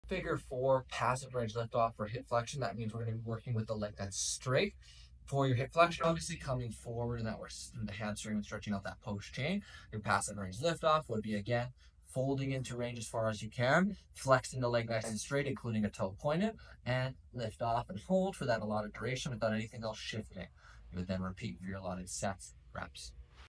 PRLO Hip Flexion Figure 4
To perform a Passive Range Lift Off (PRLO) for hip flexion in a figure 4 position, start by sitting on the floor and placing one leg in front of you with the ankle resting on the opposite thigh, forming a "figure 4." Ensure your back is straight and core engaged. Lean slightly forward to reach your passive end range for hip flexion in this position. Without using your hands, attempt to lift the figure 4 leg (the leg with the ankle resting on the opposite thigh) off the ground, engaging your hip flexors. Hold the lifted position for a few seconds, then lower with control. Focus on maintaining tension and controlled movement throughout the lift-off.